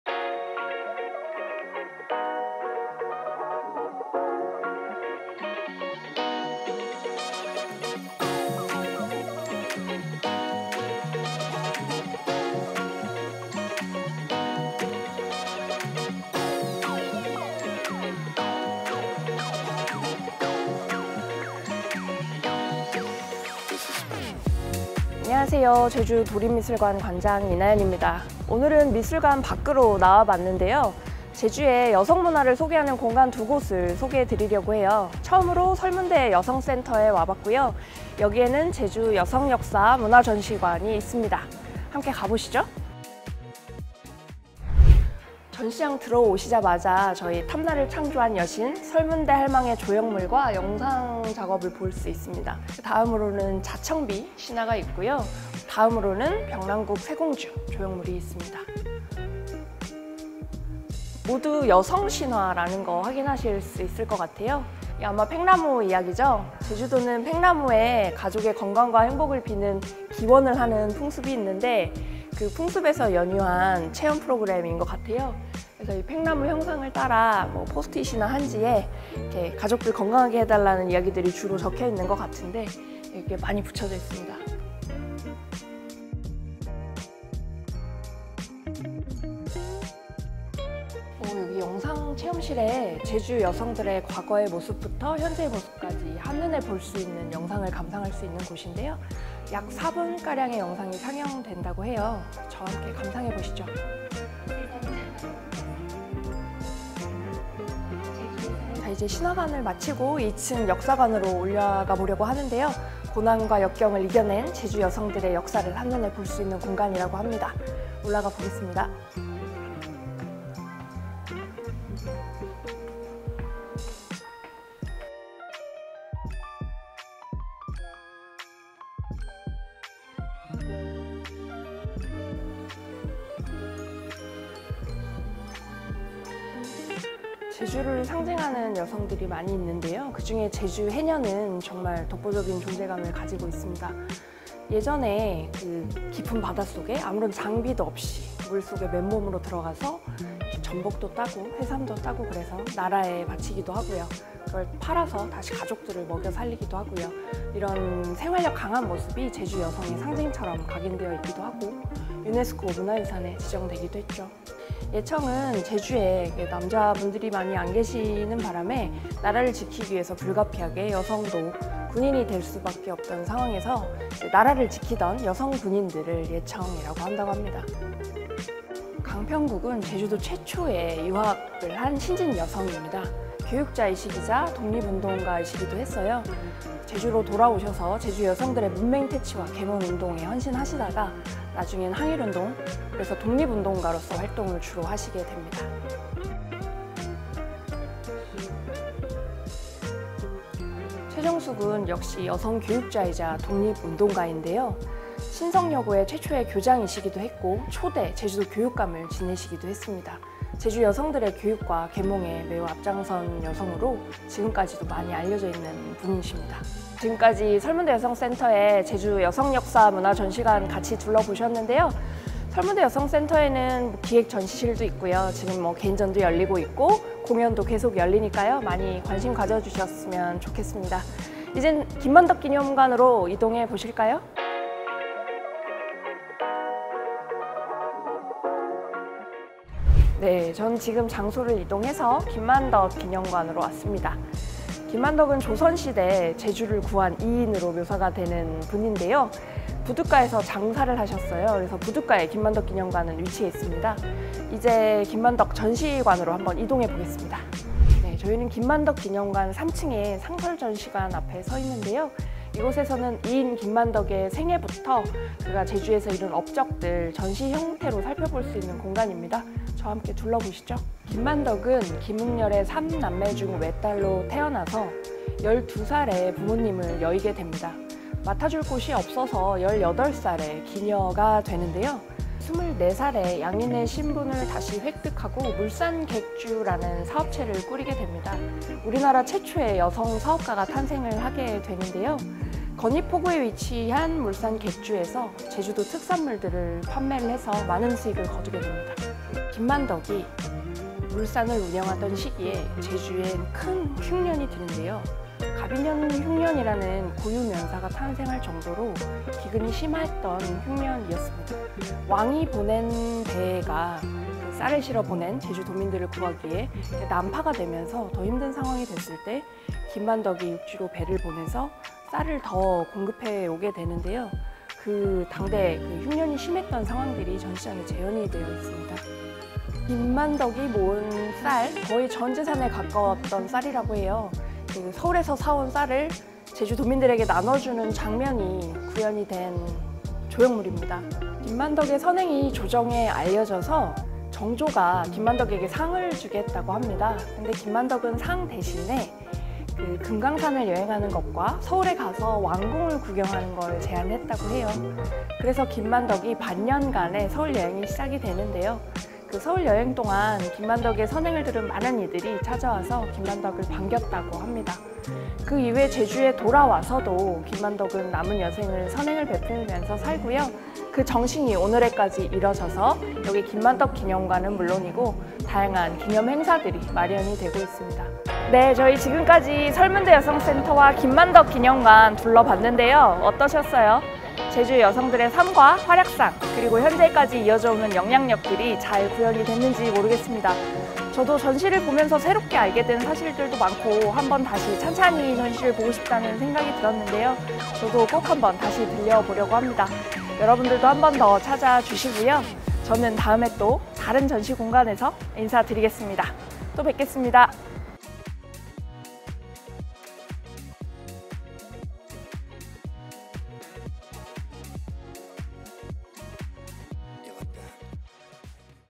제주도립미술관 이나연 관장님 밀착 Vlog [Director’s Diary_2]
Director’s Diary_2편은 제주 그리고 여성이라는 주제로 제주 시내에 위치한 설문대여성문화센터의 제주여성역사문화전시관과 김만덕기념관 2곳을 돌아봤습니다.

이번 영상에서는 탐라 창조 여신인 설문대할망. 강인한 생활력의 상징이자 제주경제를 일구는 주춧돌역할을 했던 제주 해녀, ‘화살받이'로 적으로부터 제주를 지켰던 조선시대 여성군인 제주예청, 제주선각인물인 강평국, 최정숙 등의 인물들을 살펴보며 제주 여성사의 보석 같은 발자취를 영상에 담아보았습니다.

조선시대에 '여성' 과 '기녀', '지방'이라는 한계를 이겨내고 성공한 우리나라 최초의 여성 CEO이자, 극심한 흉년에 전 재산을 내놓아 백성들을 구한 김만덕의 도전정신과 나눔의 삶의 발자취를 Director’s Diary2편에 담았습니다. 여러분의 많은 시청 부탁드립니다. :)